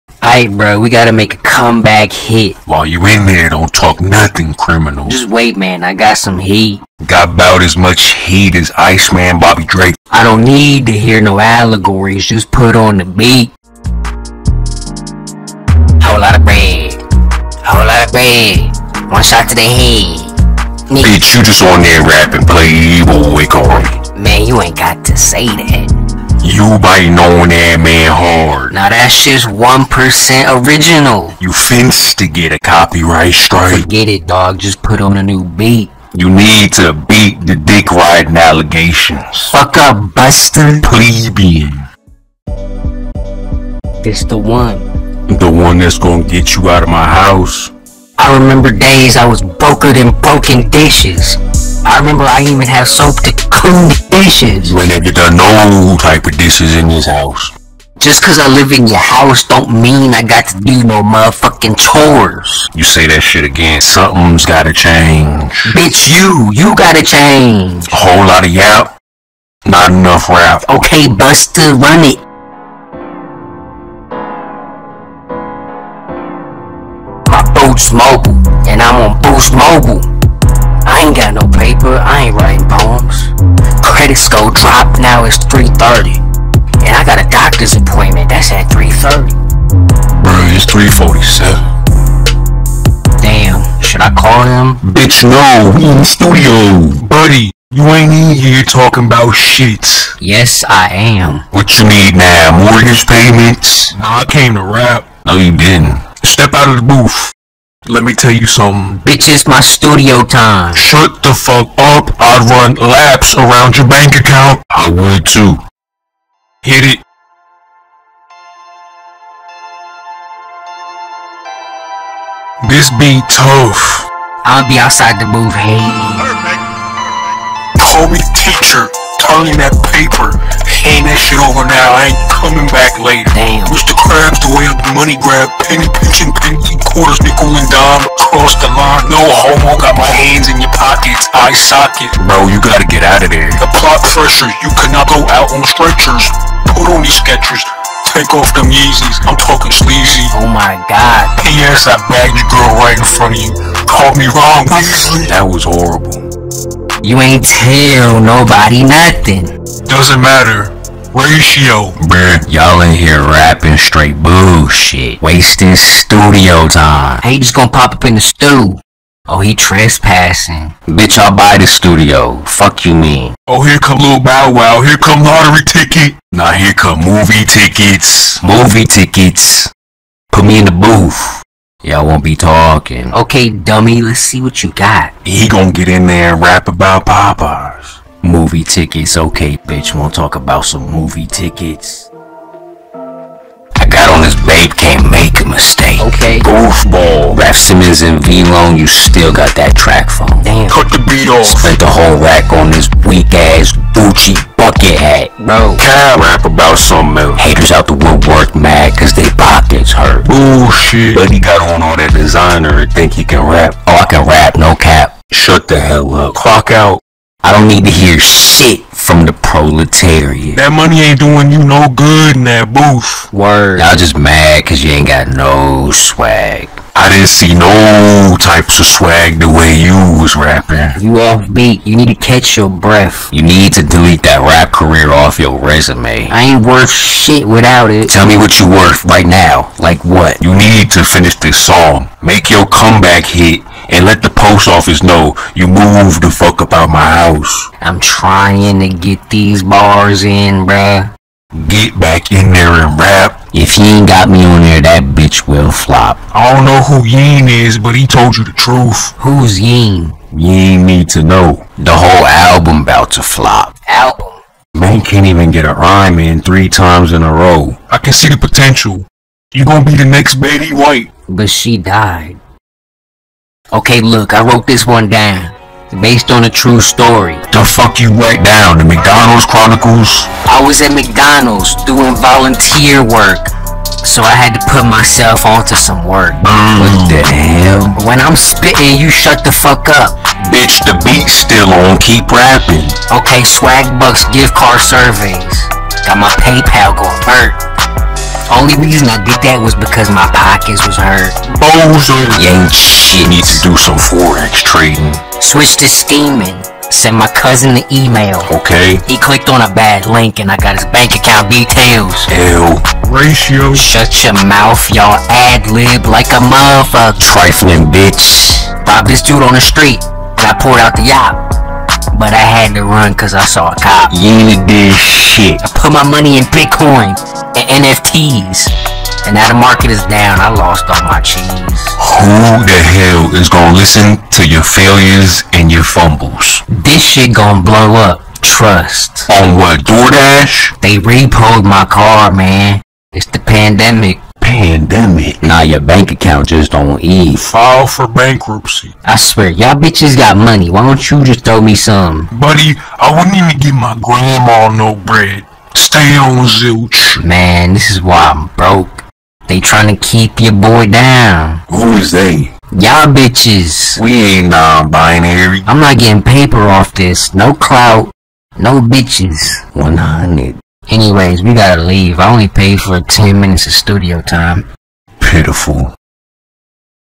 Studio Time [Baku Season 6]
THEY BACK IN THE BOOTH!! Yung Cash Register is once again trying to make music. Trying quite hard. Still not getting his paper. The way Baku lets Lil Broomstick talk to him like that still is crazy.

Baku Season 6